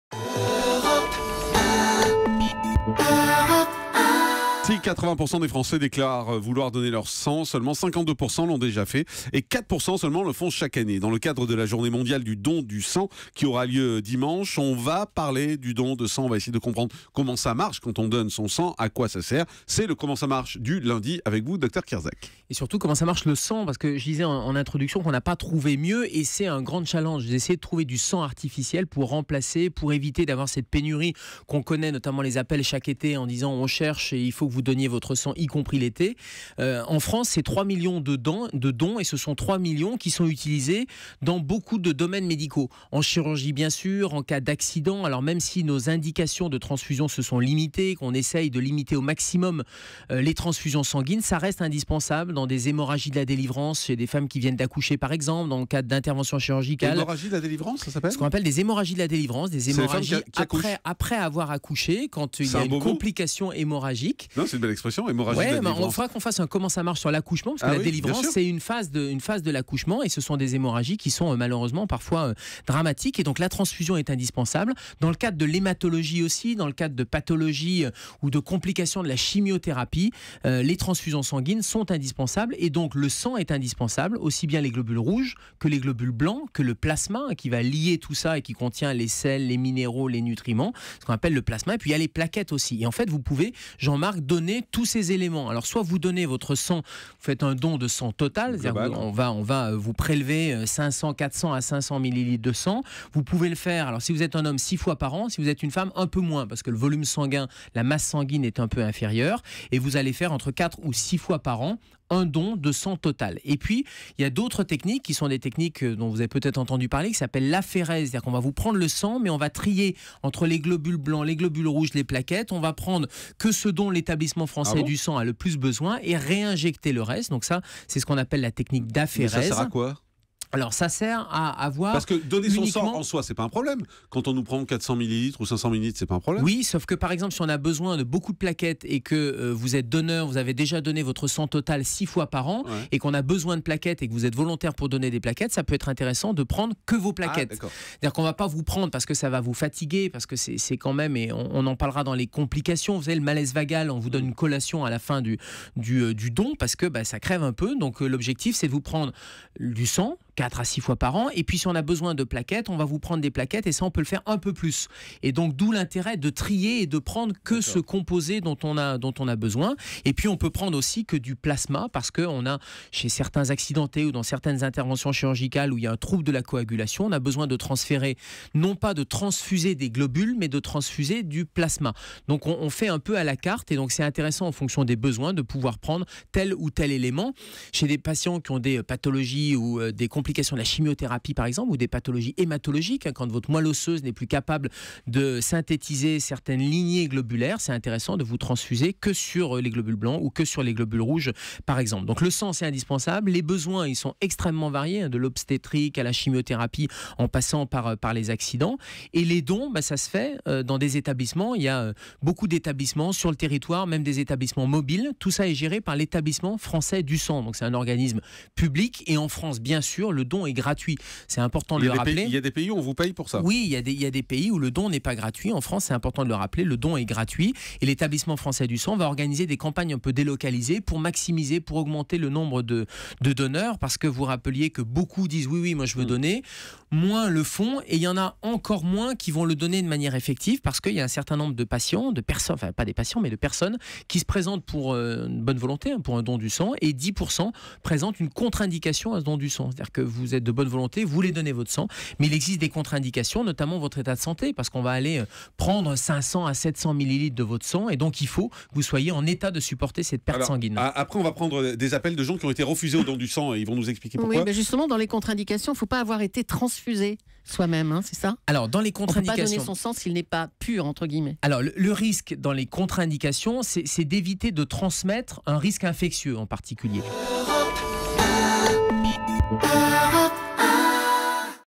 Comment ça marche : le don de sang ?
Gérald Kierzek nous explique comment fonctionne le don du sang.